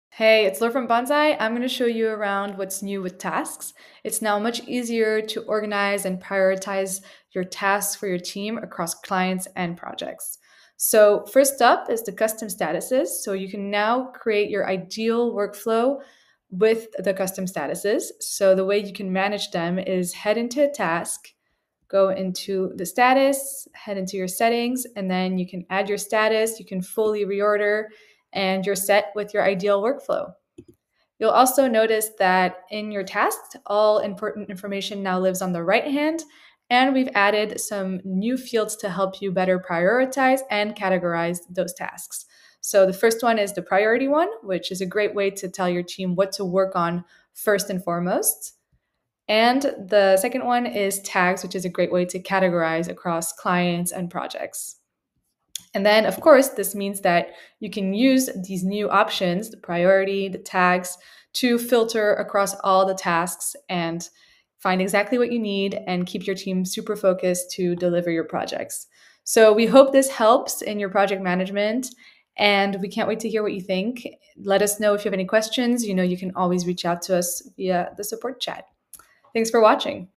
How to Organize & Prioritize Tasks Effectively with Bonsai
Learn how to organize and prioritize your team's tasks more efficiently with Bonsai's new task management features. Designed to help teams streamline workflows across clients and projects for better productivity.

This video will cover:
✅ Creating custom task statuses to build your ideal workflow.
✅ Using priority settings to ensure your team works on the most important tasks first.
✅ Adding and managing tags to categorize tasks across clients and projects.
✅ Filtering tasks by priority and tags to stay organized and focused.
✅ Enhancing project management by keeping all important task information in one place.

🚀 Book a free demo with Bonsai and learn how we can help you scale your business